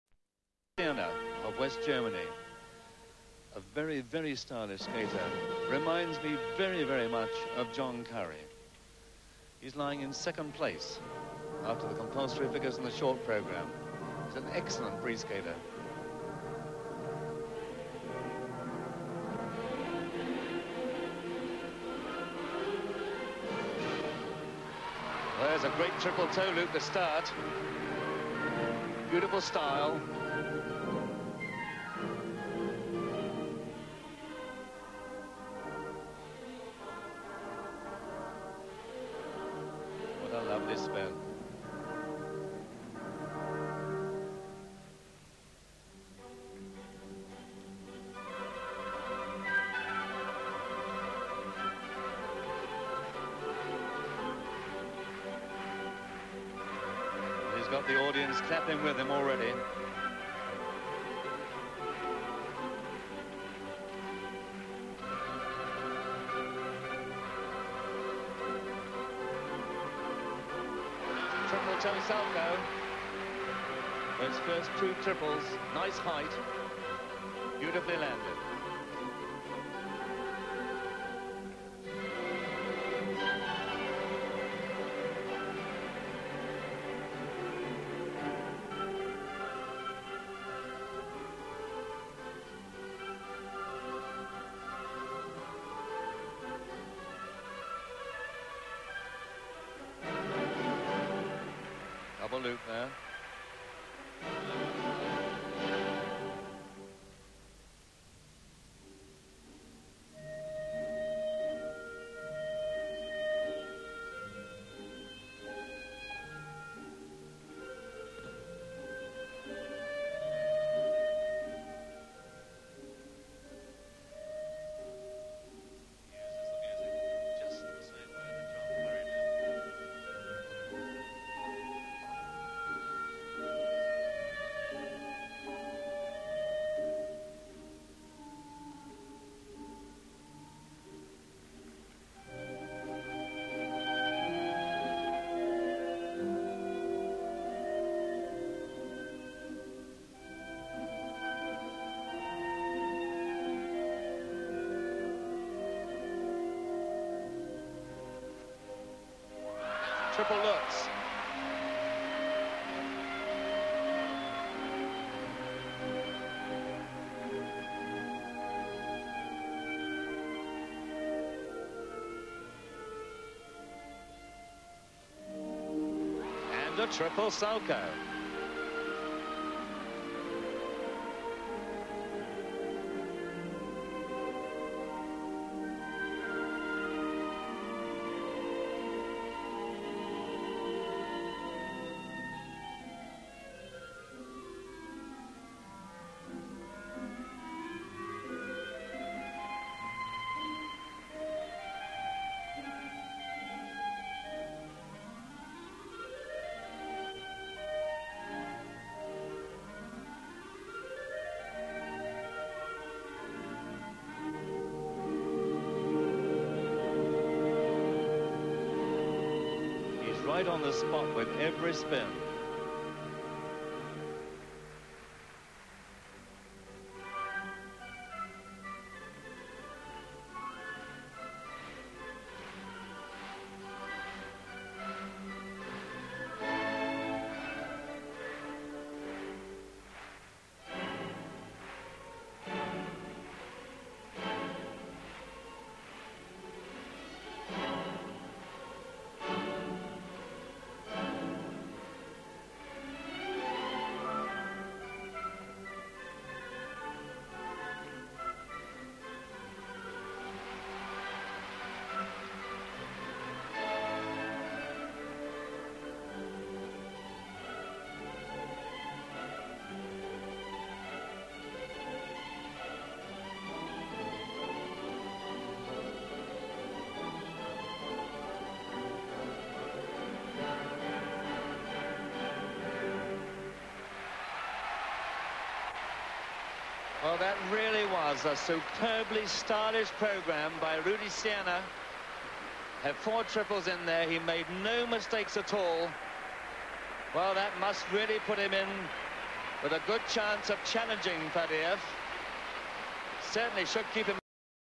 Rudi Cerne LP Europeans 1984
I love his use of the music here - very similar in style to John Curry